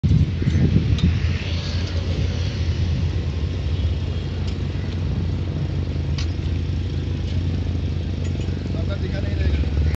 Street food Mumbai
People love street food like bhelpuri, pani puri, shev puri, ragada pattice,etc in Mumbai. It is not hygienic but very tasty. People from all over India and some foreigners love this food